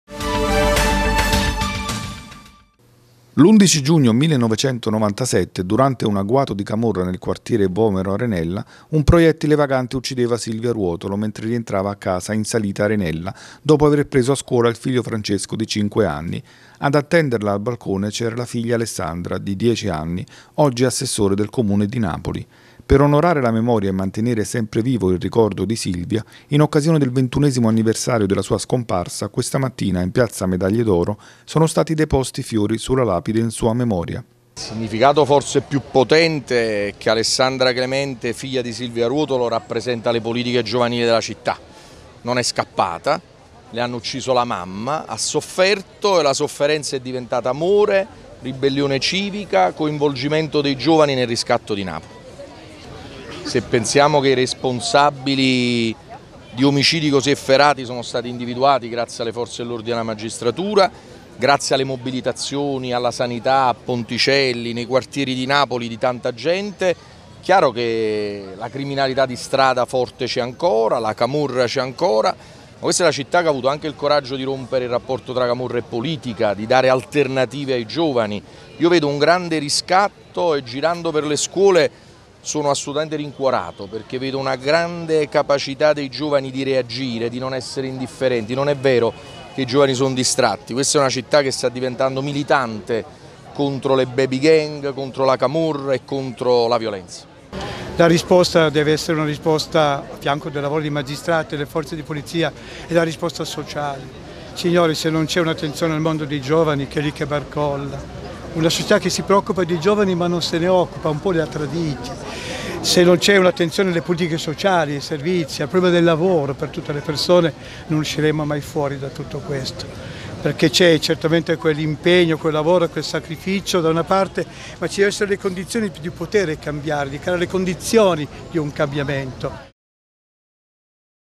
Silvia Ruotolo, il ricordo e l'eredità quotidiana
21 anni fa moriva Silvia Ruotolo, per colpa di un proiettile vagante in un agguato di camorra. Oggi la figlia Alessandra Clemente è Assessore del Comune di Napoli. In piazza delle Medaglie d'Oro il ricordo e la mobilitazione di cittadini ed associazioni.